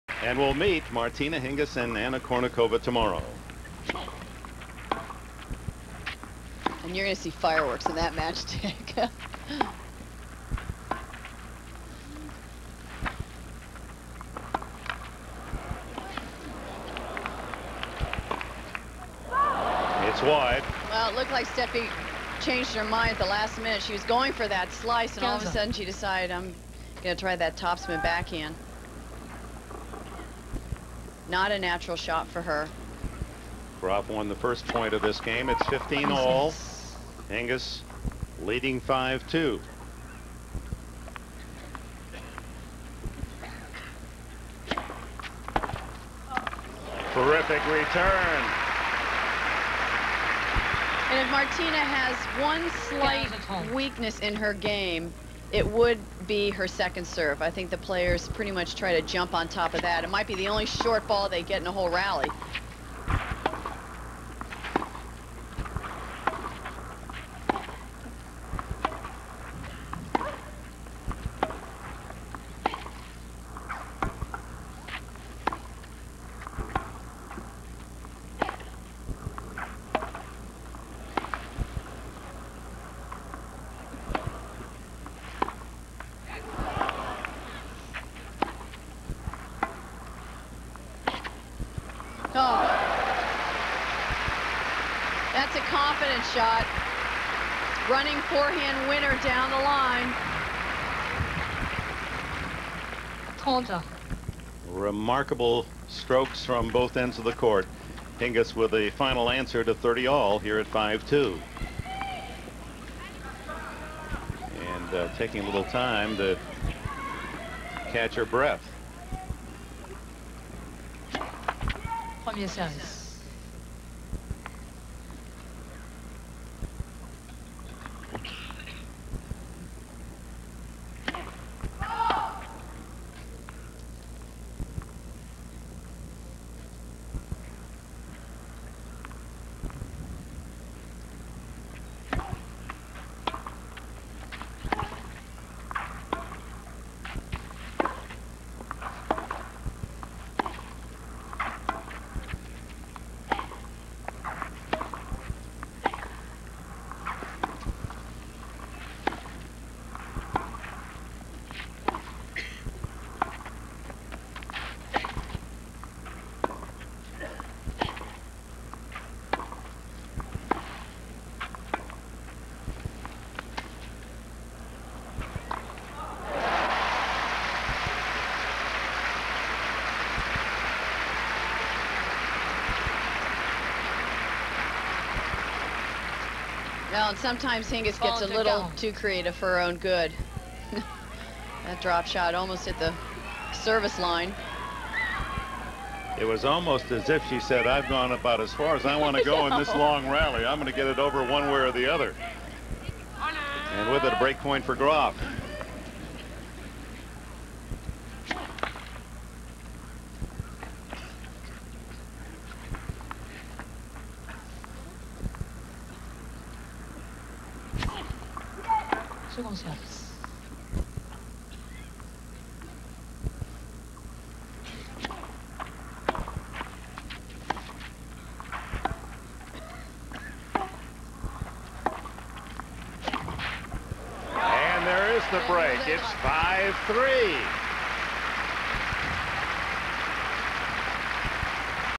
Steffi Graf (Hingis Serving for 1st set of '99 French Open Final)
Steffi Graf:  In the final of the '99 French Open, Hingis struggled to serve out the first set and the match.  She lost her service game the first time she tried to serve for the first set.  And, of course, she could not serve out the second set and would go on to lose that set and the match.  This was a problem Hingis had with a couple of opponents, including Graf.  For another example of this problem, see the clip titled "Steffi Graf - Rivalry with Hingis."  French 1999.